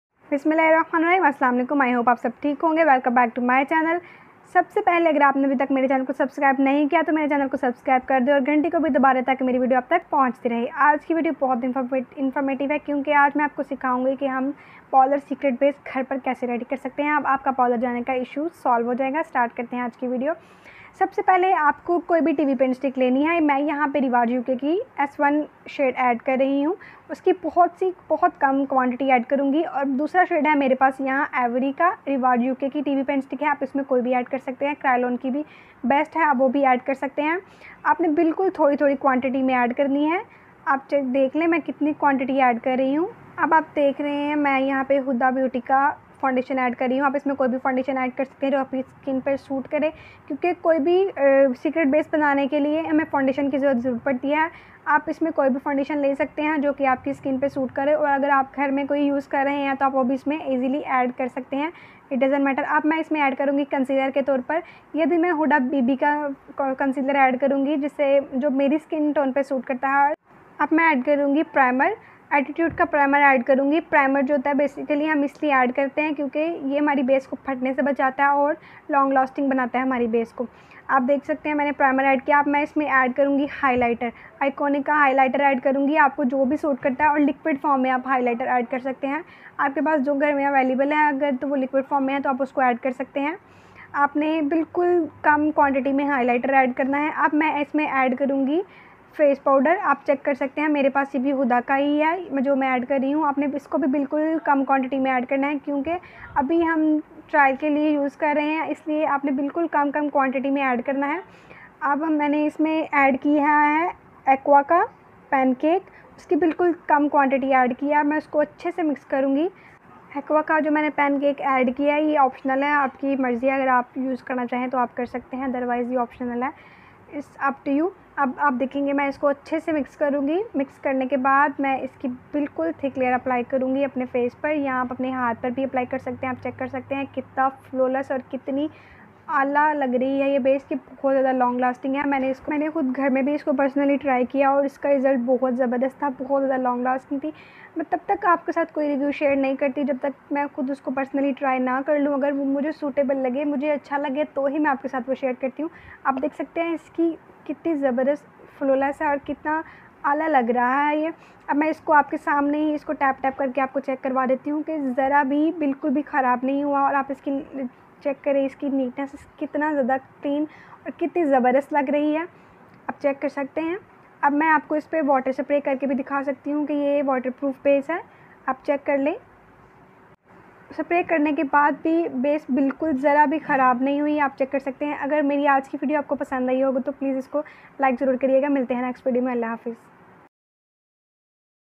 How to Make a Parlor Homemade Customize Base for Beginners - Perfect for Summer Makeup
Parlor homemade customize base for beginners/parlor secret  customize base for summer perfect Makeup base